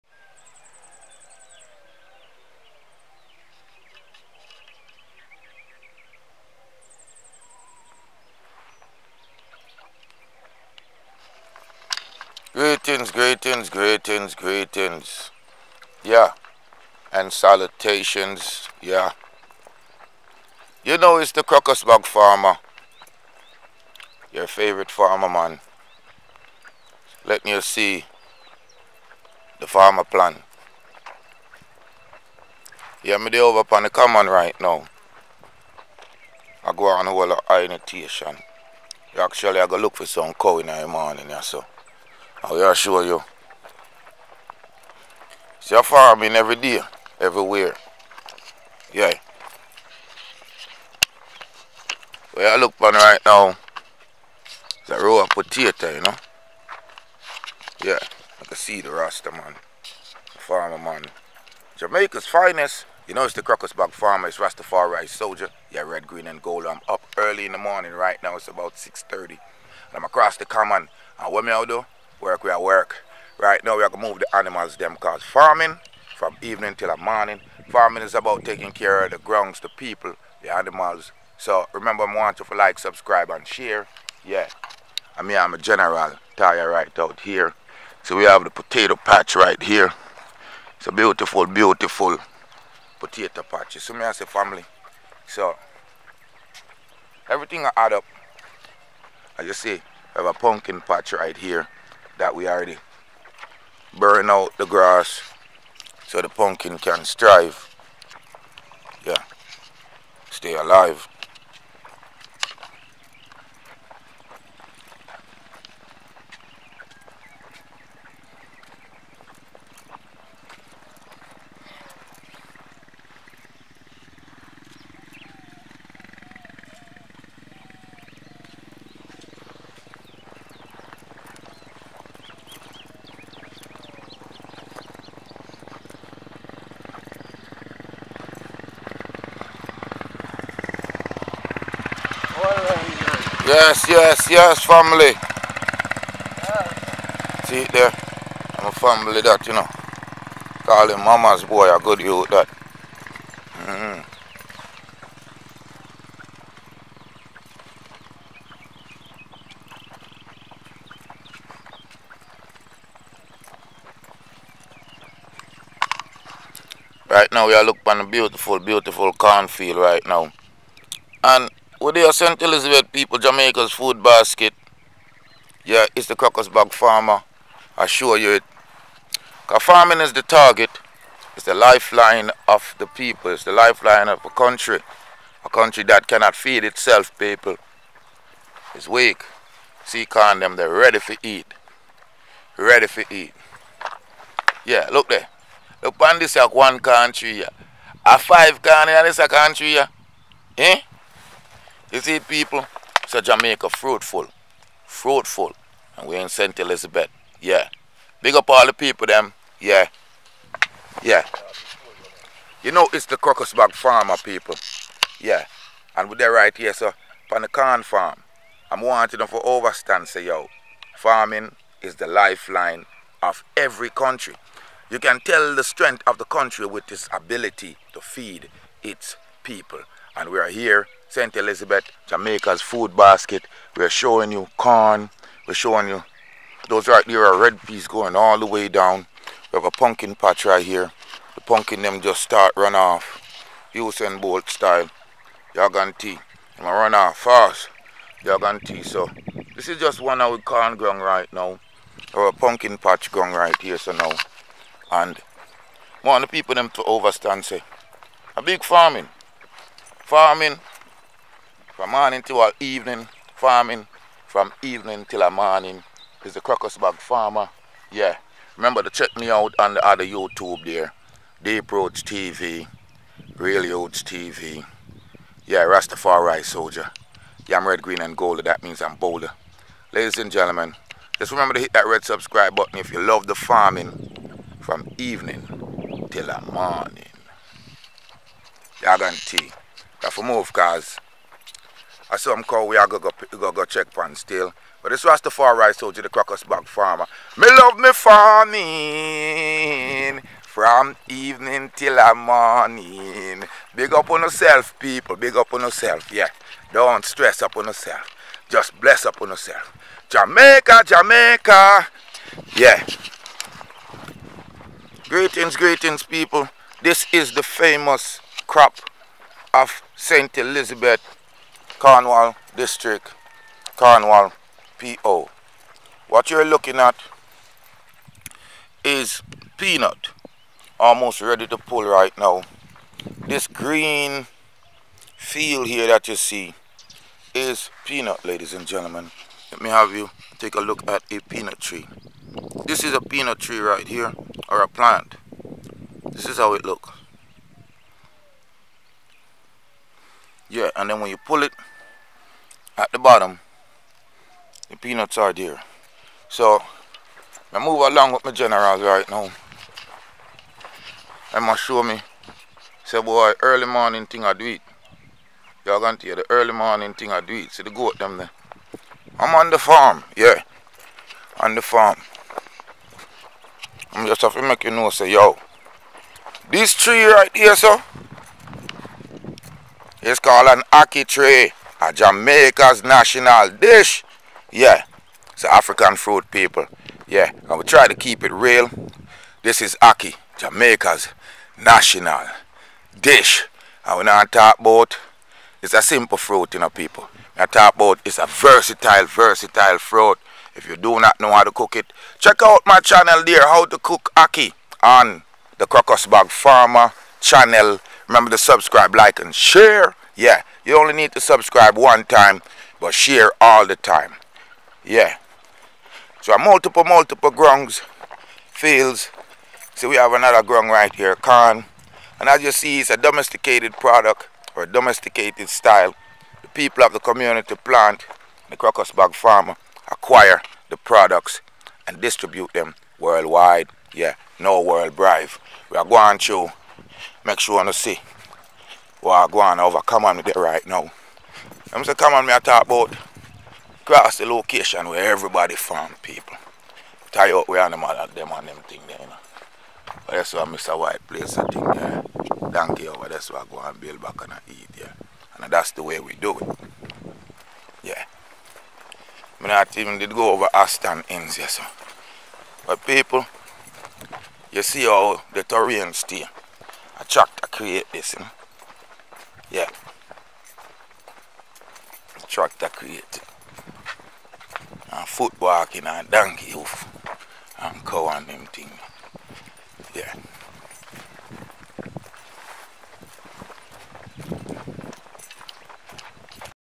Big Farm st elizabeth Jamaica Rasta tour | Rasta Farming | The Crocus Bag Farmer
Big Farm Jamaica Rasta Man tour Big Farm in the Island. TheCrocusBagFarmer FarmingRasta RastafariSoldier BigFarm Farming Rasta Man St Elizabeth